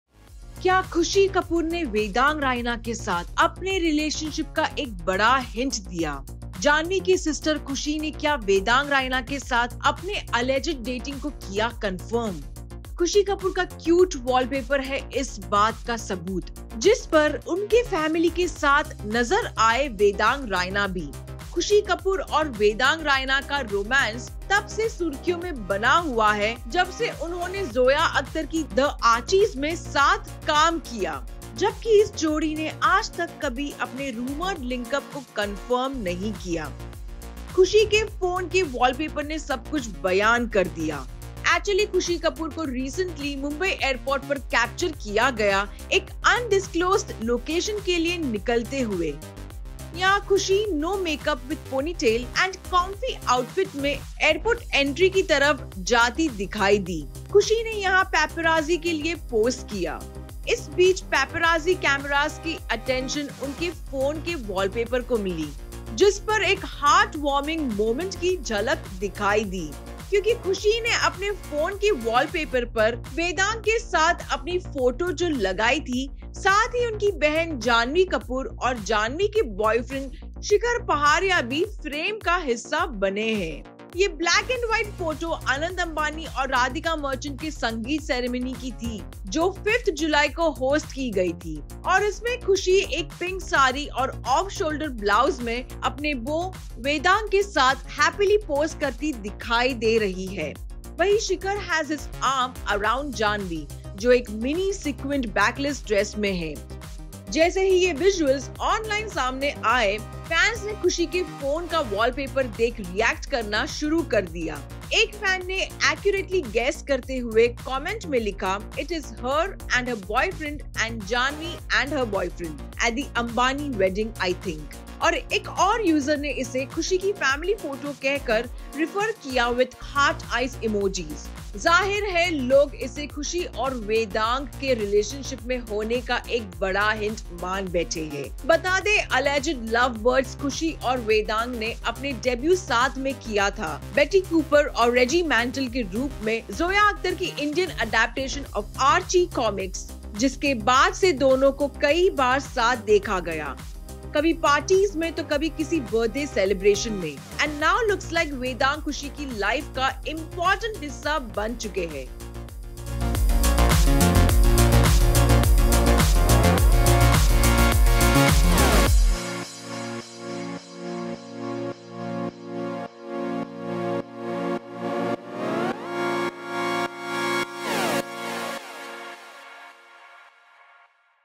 Khushi Kapoor CONFIRMS dating rumoured BF Vedang Raina with her cute wallpaper?
Bollywood actress Khushi Kapoor made her debut in the Hindi entertainment industry with Zoya Akhtar’s film The Archies. Despite the movie receiving a lot of negative reviews, she won over the audience with her charming personality. In recent months, there have been persistent rumours about her being in a relationship with her co-star Vedang Raina. Although she has denied these rumours in interviews, it appears she has recently given a significant hint about their relationship. Watch the video to learn more.